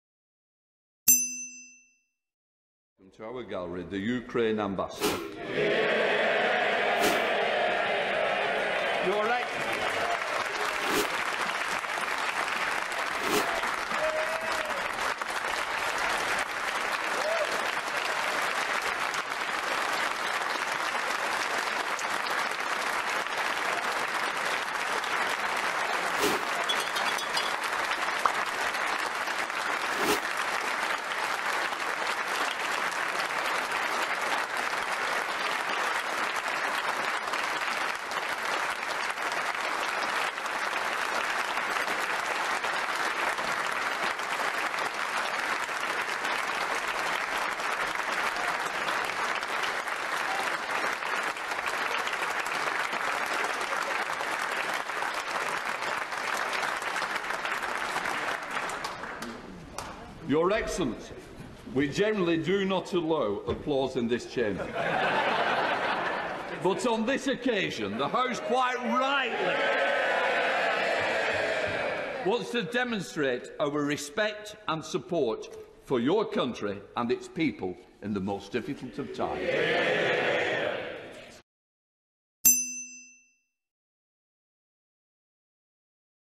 UK Parliament honour the Ukrainian people and Ambassador
A rare show of unity in the UK Parliament with every single member across all parties (bar none) taking to their feet for a standing ovation for the Ukrainian Ambassadors presence.